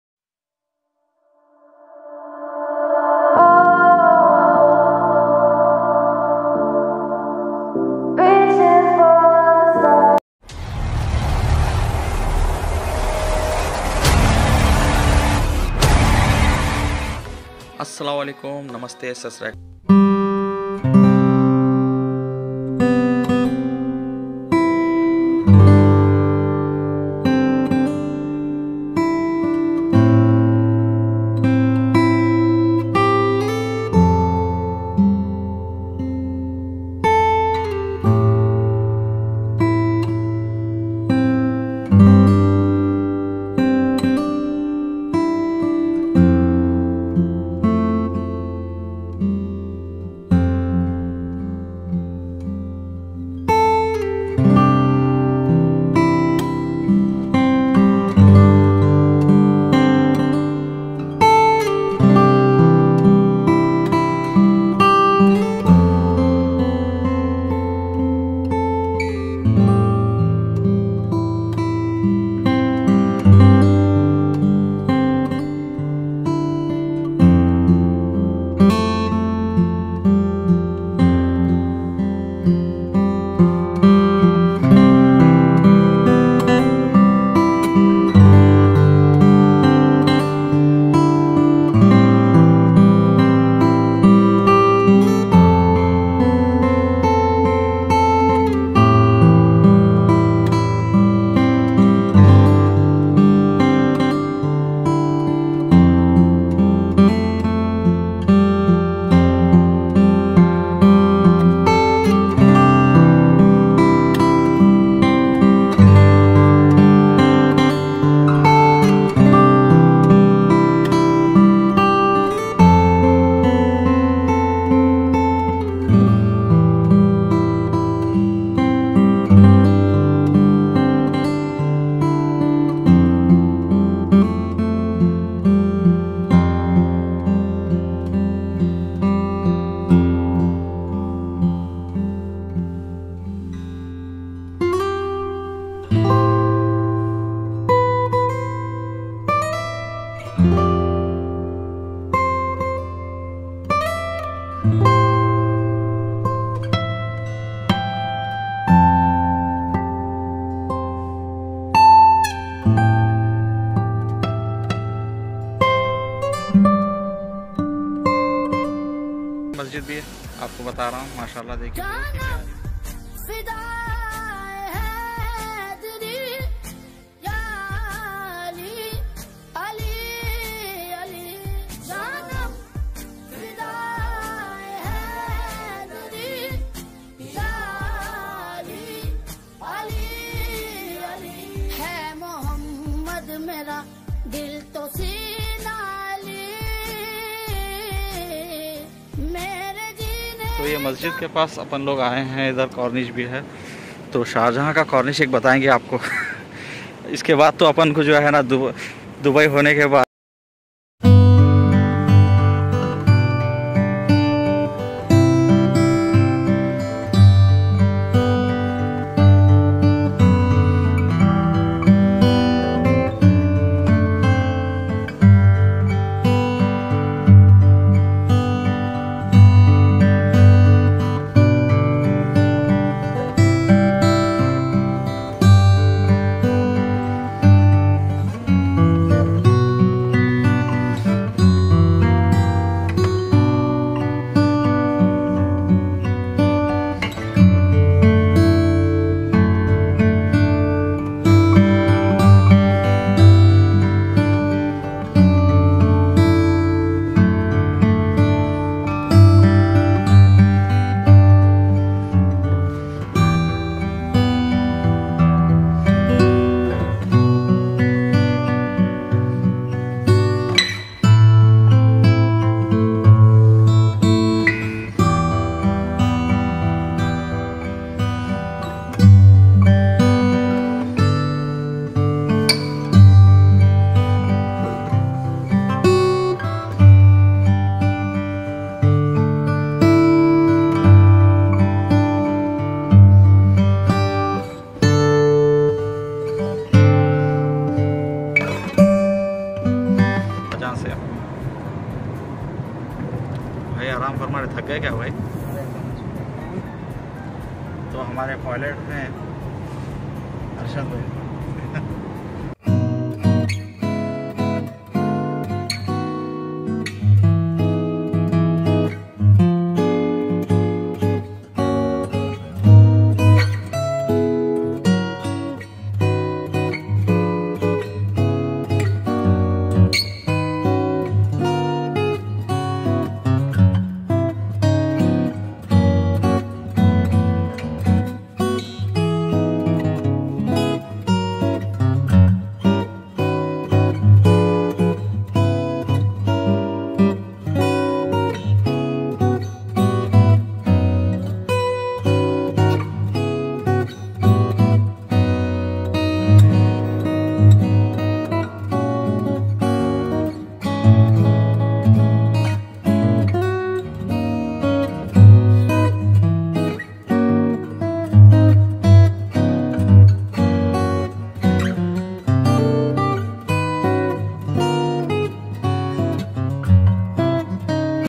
DUBAI TO SHARJAH BY CAR | VLOGS-5 | MOSQUES | CORNICHE | SHEIKH ZAYED ROAD | SHARJAH MUNICIPALITY
DUBAI TO SHARJAH BY CAR | VLOGS-5 |   MOSQUES | CORNICHE | SHEIKH ZAYED ROAD | CAFETERIA | SHARJAH MUNICIPALITY | ZEESHANSIDDIQUI_VLOGS |

LIKE | COMMENT  | SHARE |  N  | CLICK ON OUR BELL 🔔 ICON  FOR  NEW VIDEOS.